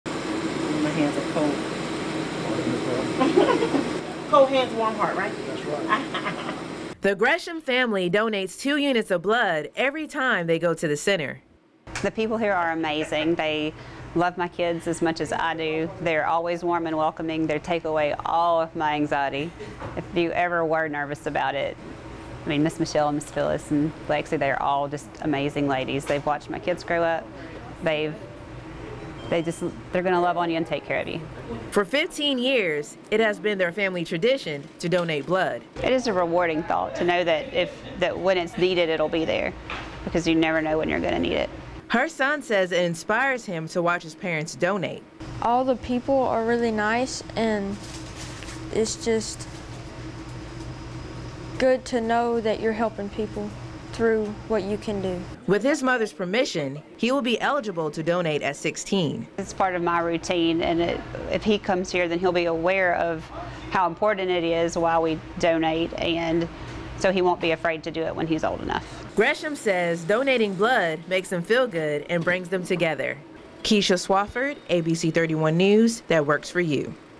Gresham Family Donates Blood as a Family Tradition to Save Lives
According to the American Red Cross, every two seconds someone in the U.S. requires a blood transfusion. ABC 31 News Reporter Keisha Swafford has the story on why a family donates blood every year.